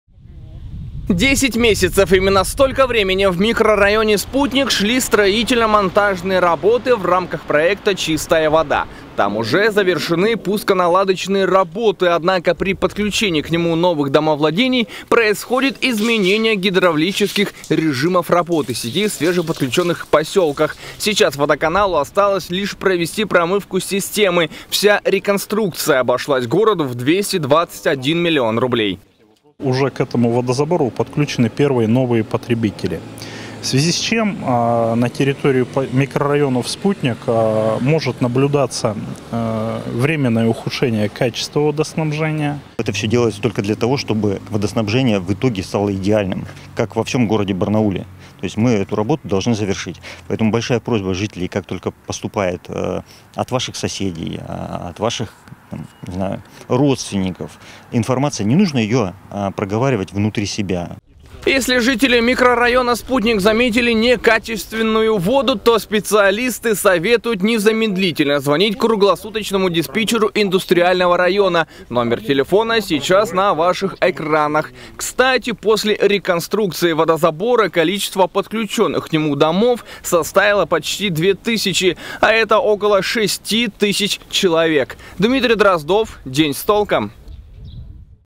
"Чистая вода". В микрорайоне Спутник завершилась реконструкция артезианского водозабора
Сейчас идет промывка систем, а это временно отразится на качестве воды. Что делать местным жителям, если они заметили, что вода изменила цвет и вкус?